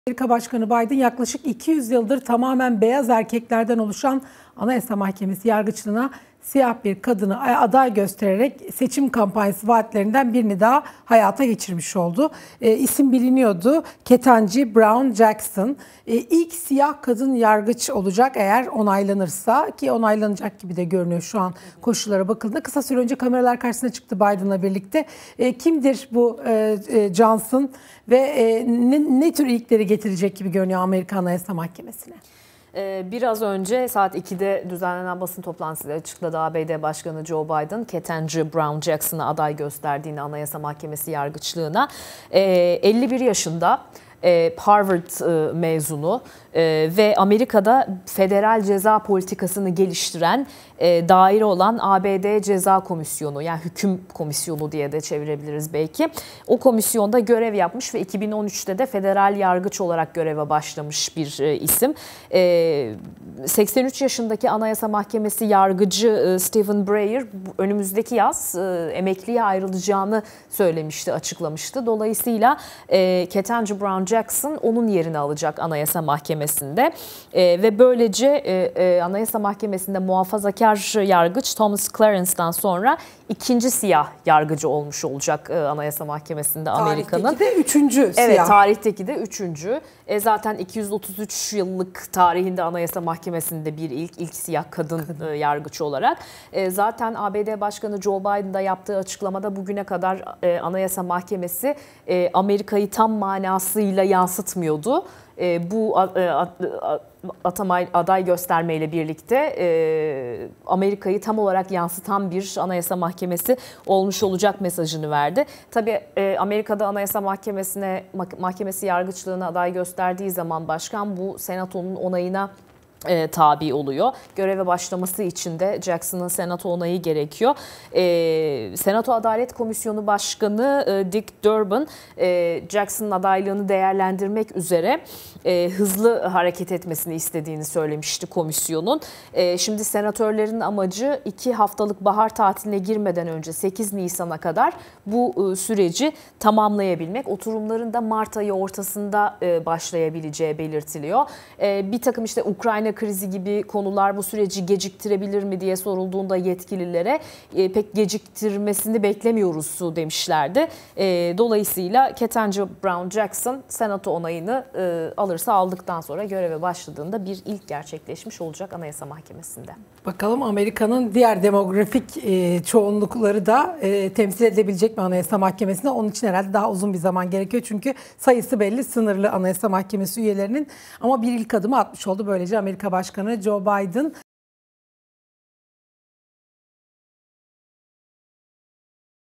ABD Anayasa Mahkemesi'nde Bir İlk
ABD Başkanı Joe Biden bir ilke imza atarak Anayasa Mahkemesi yargıçlığına Ketanji Brown Jackson'ı aday gösterdi. Jackson, Anayasa Mahkemesi yargıçlığı için seçilen ilk siyah kadın.